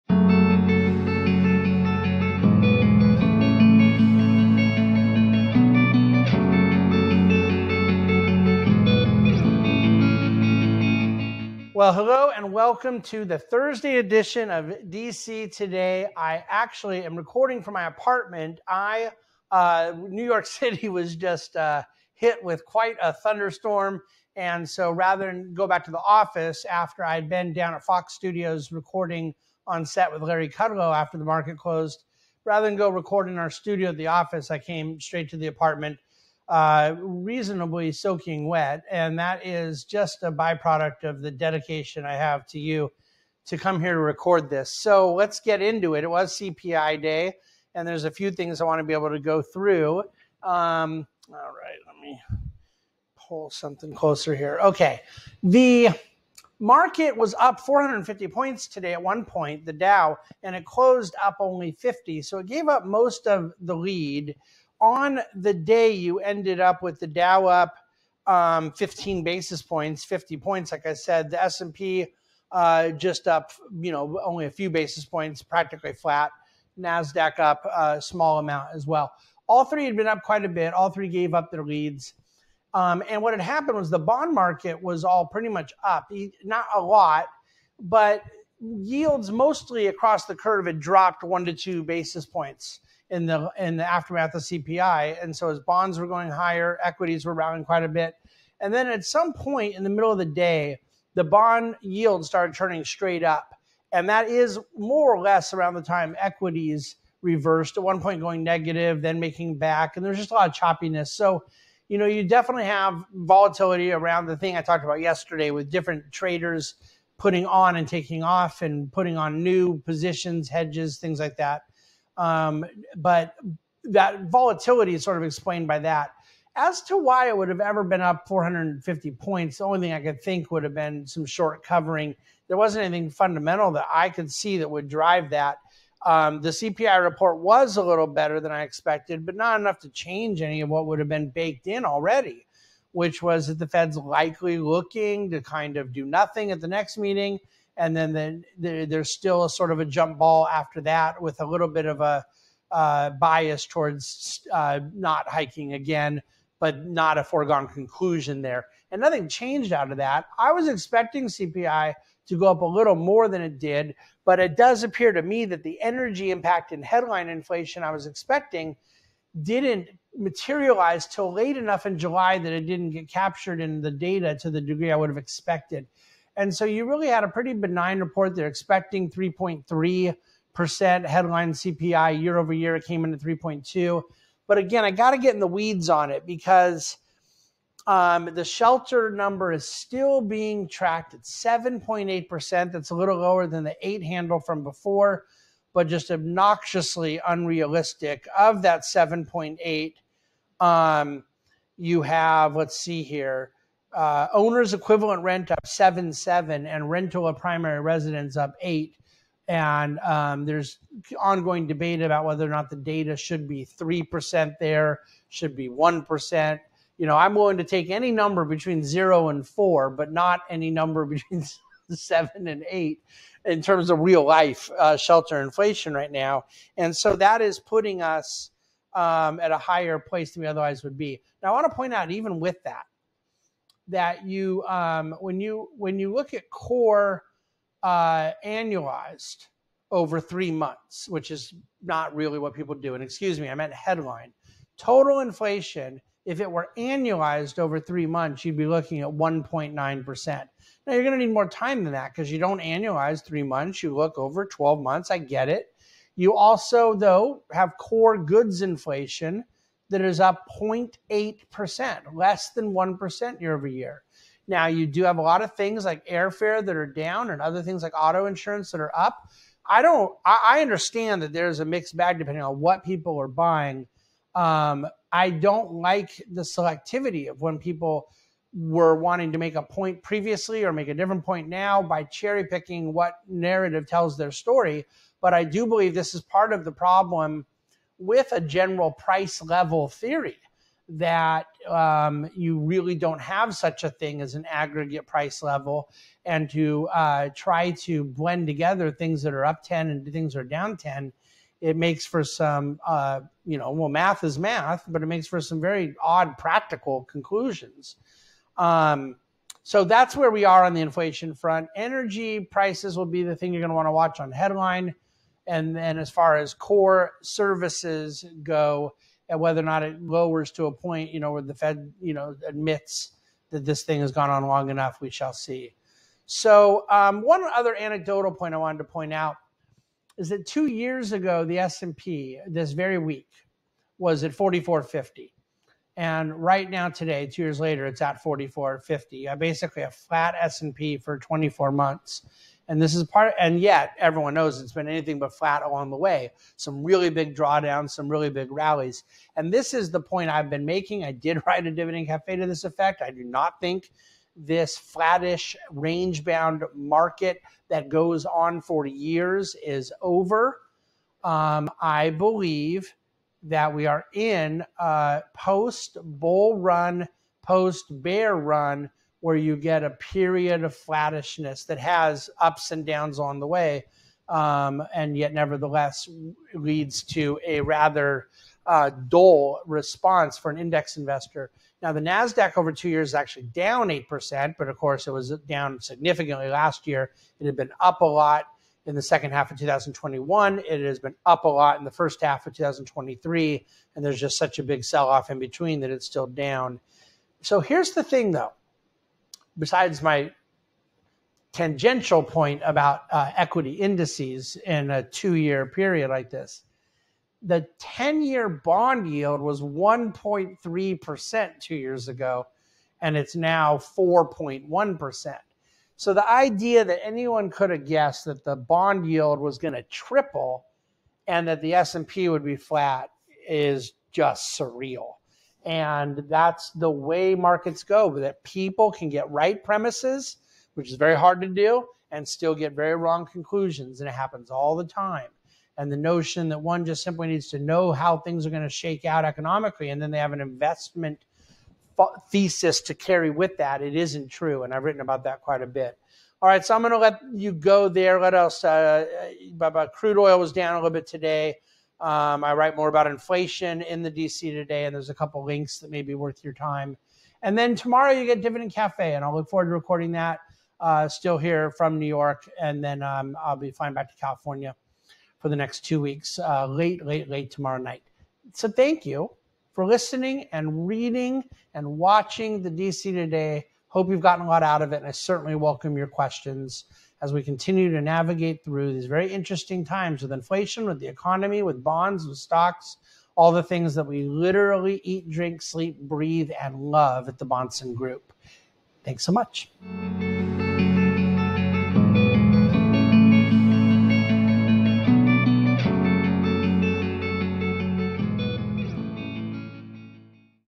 The DC Today - Thursday, August 10, 2023
A tad to my surprise CPI came in even lower than expected (+3.2% vs. consensus estimates of +3.3%).  The core rate was +4.7%, as anticipated.  On the month, prices ex-food, ex-energy and ex-the B.S. shelter figure were down -0.1%.  Within that +3.2%, the shelter component was up +7.8%, as the model shows OER (owner’s equivalent rent) up +7.7% on the year and Rentals of primary residence up +8%.  Uh huh.